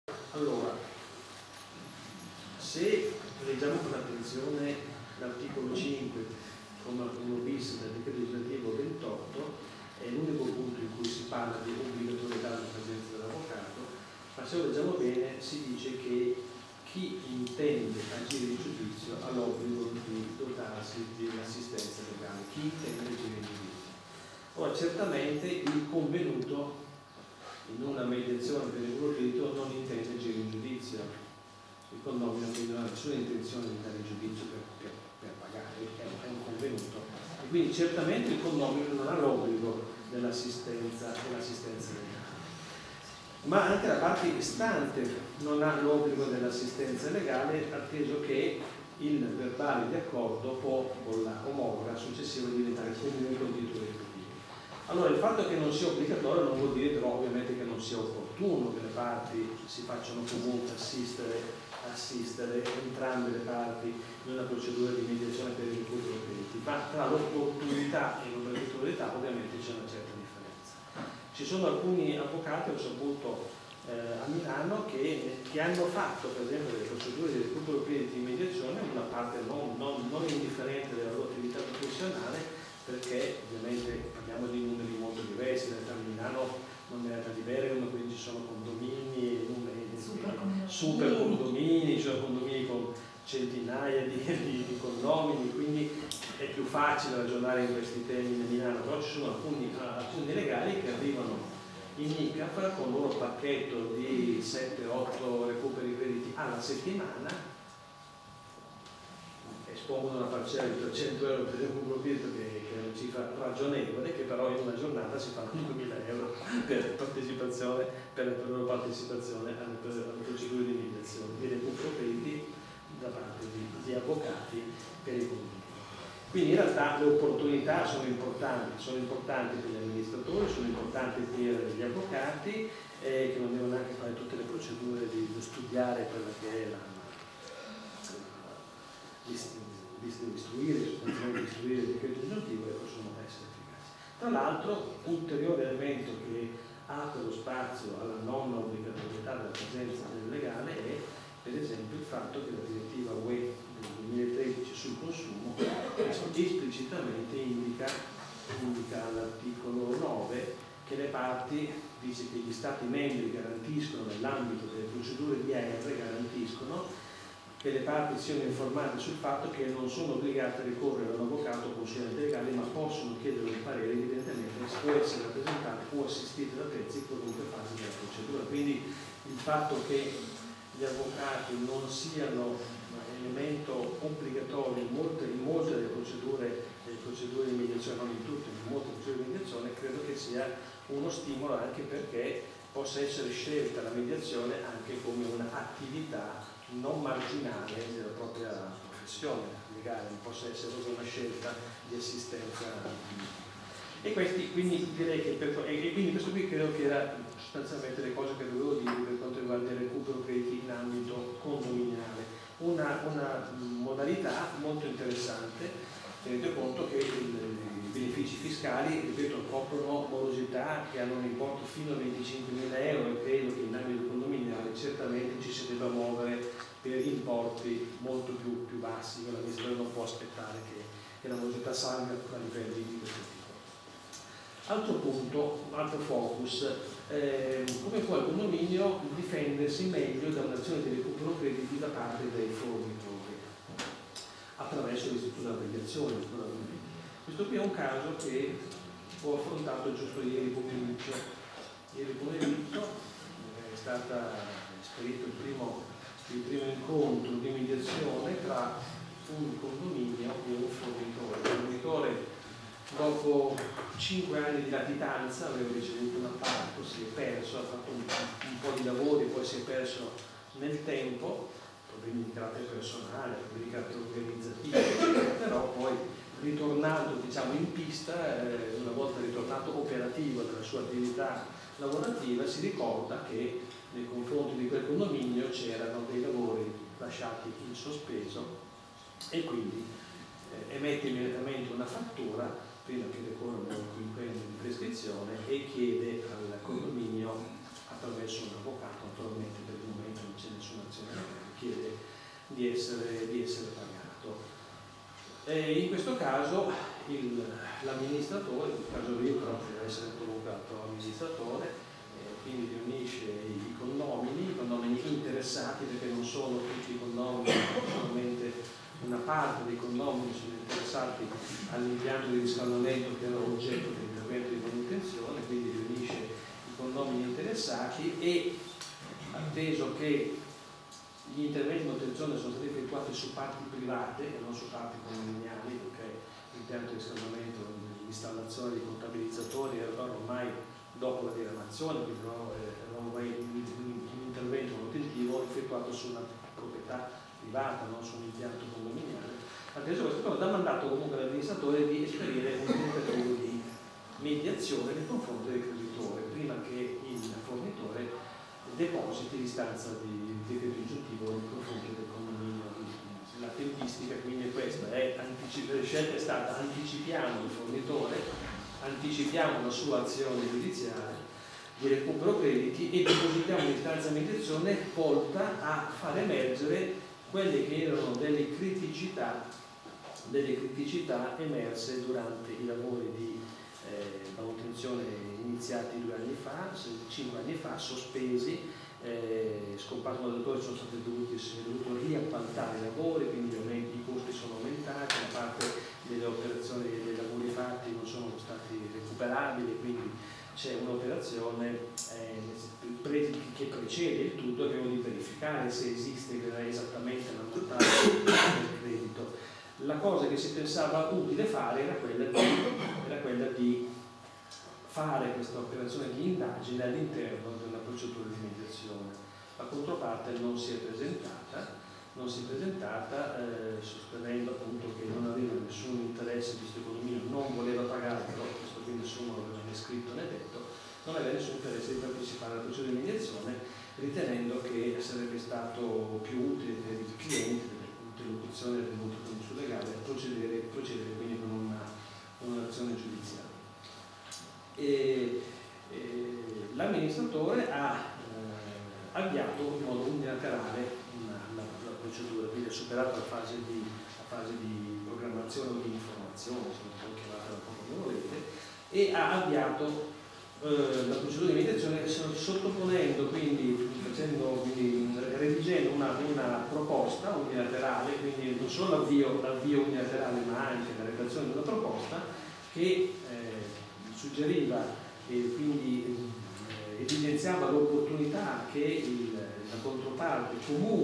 Recupero crediti e mediazione 5/19
SEMINARIO
IL RECUPERO CREDITI TRAMITE IL PROCEDIMENTO DI MEDIAZIONE CIVILE
Evento organizzato dallo Sportello di Conciliazione ICAF di Bergamo – Mutuo Soccorso
martedì 18 dicembre 2018 – orario 14:45 – 18:00
Mutuo Soccorso (sala convegni) – via Zambonate 33, Bergamo
Avvocati n.3 crediti, Geometri, mediatori ICAF n.10 crediti , revisori AIReC n.10 crediti
ingresso libero e gratuito